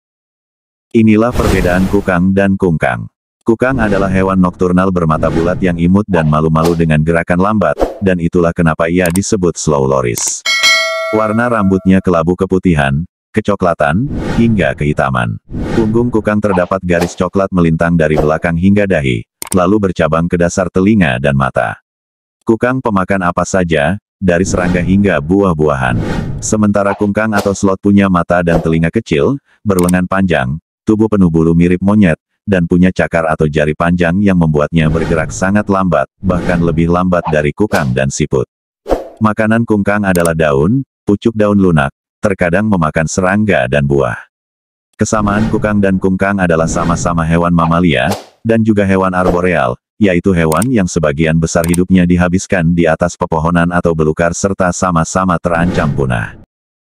Kukang dan Kungkang
Yang masih bingung antara Kukang atau Slow Loris dengan Kungkang atau Sloth, hewan yang jalannya sama sama lambat ini bisa tonton video ini agar lebih mengerti.